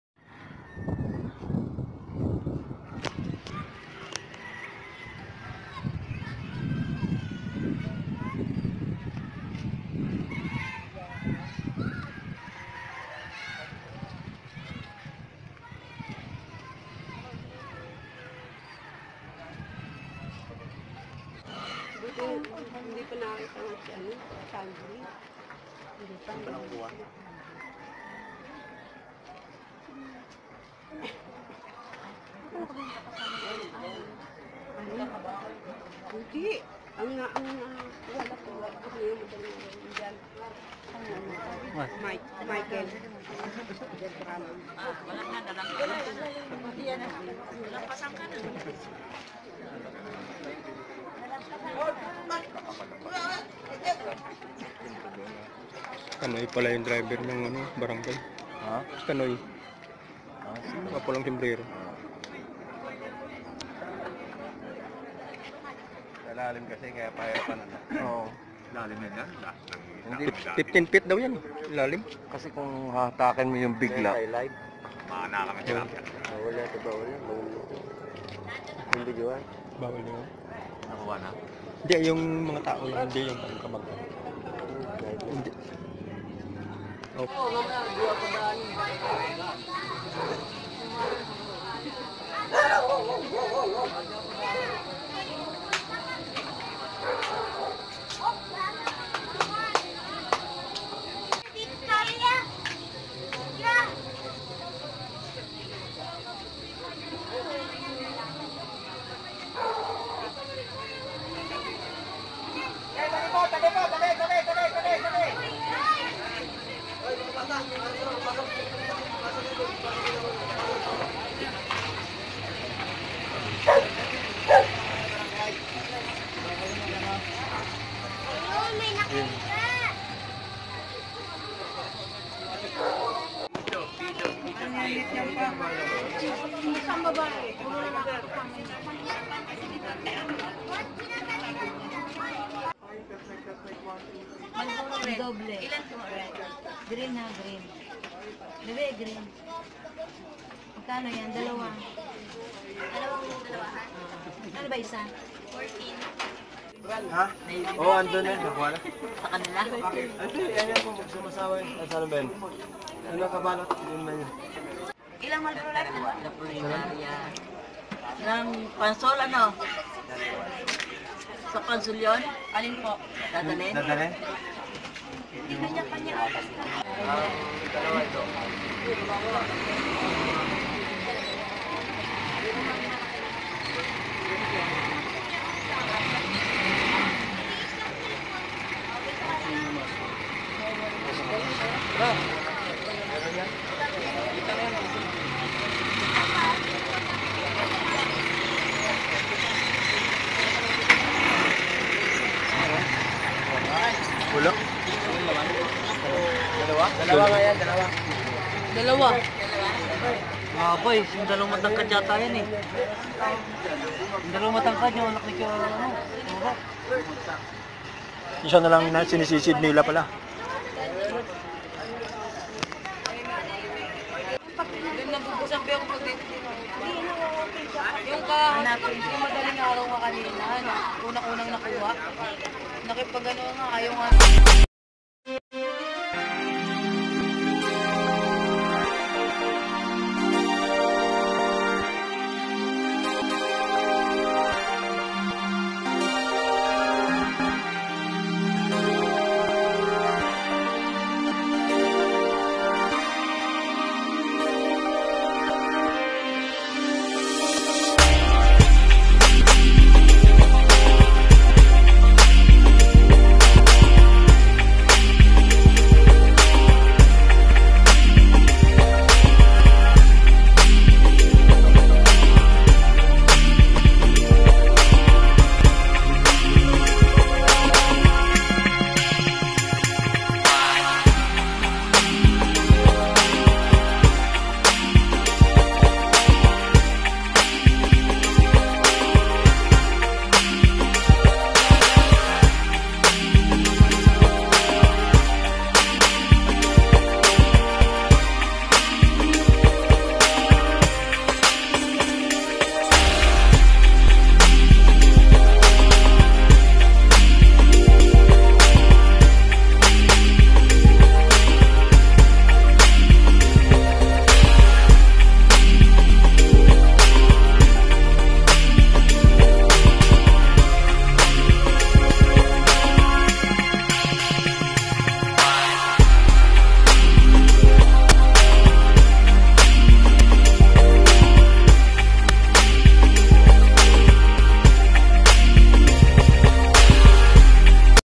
ANG PAG AHON SA MGA NASAWI ( 2nd part)
ang pag ahon sa mga biktima
ang unang apat na cadaver
ang dalawa visible na
may isa pang natira